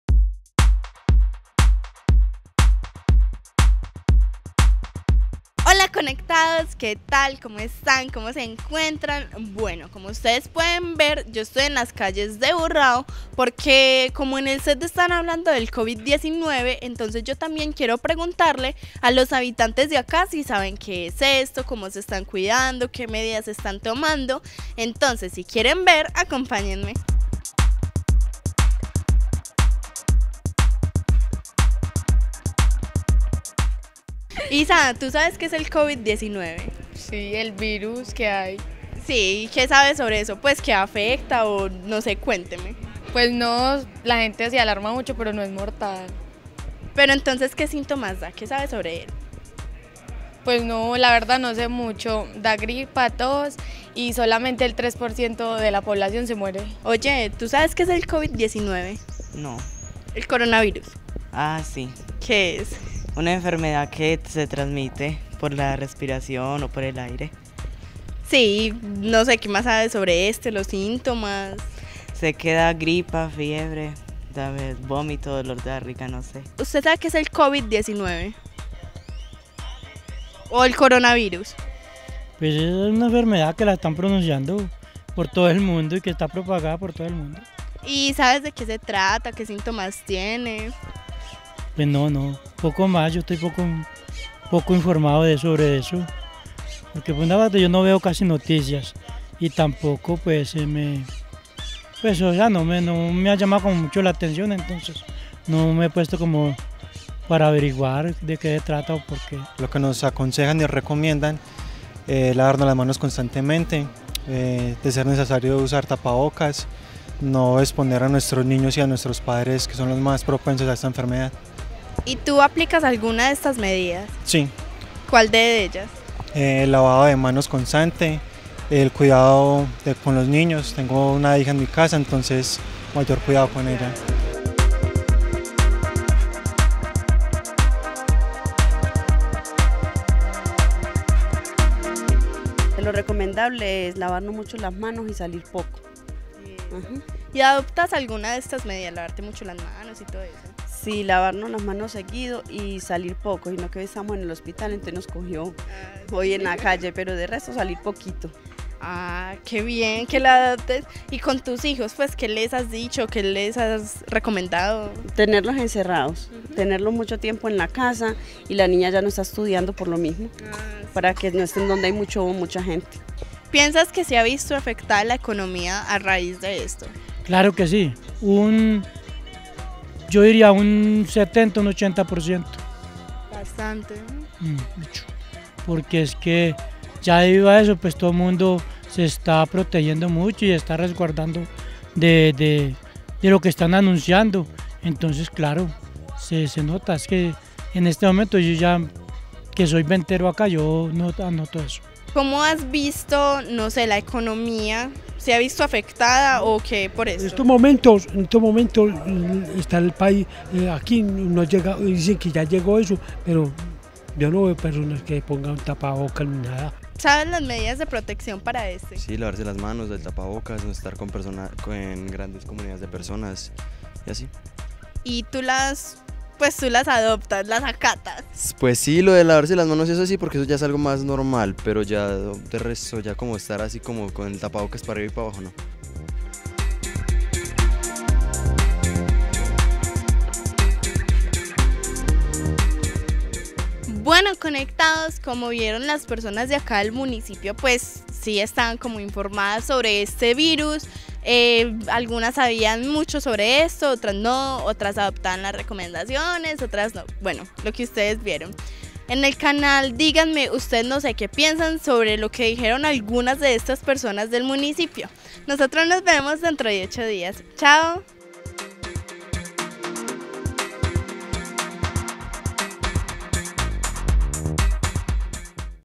Conectados Con: ¿Qué es el COVID-19 y Qué cuidados estan tomando en Urrao?
Salimos a las calles de nuestro municipio a mirar cuánto saben los urraeños acerca del COVID-19 y qué cuidados y medidas estan tomando para prevenir posibles contagios...